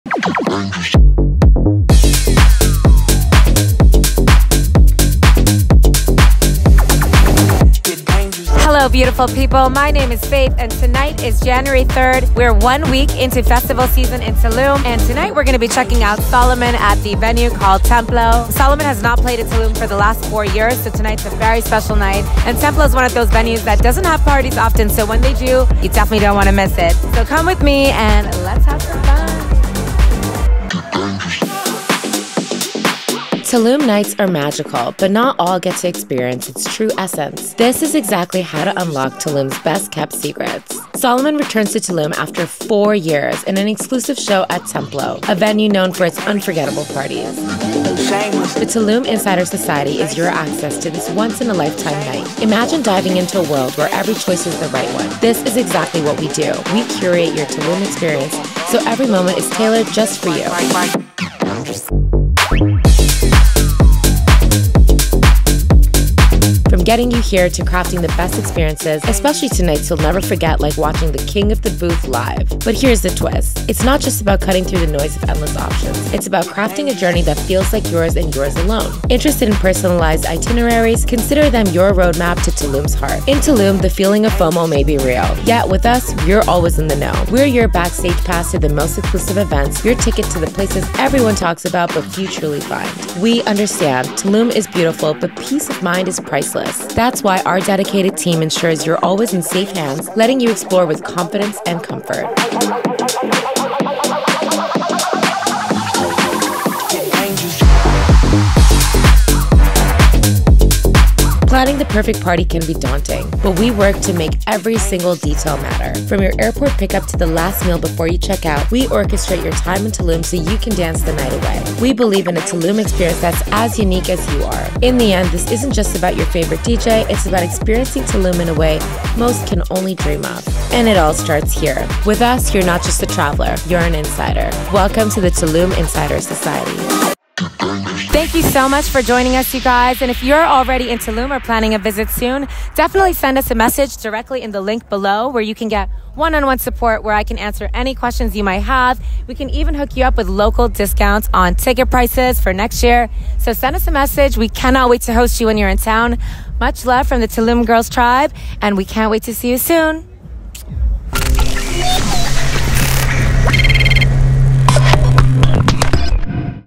We Survived Solomun Tulum 2024 With This Insider Tips!
Get the insider tips on how we survived Solomun Tulum 2024! From accommodation to finding the best spots, this video has everything you need to know before heading to one of the hottest festival parties of the year.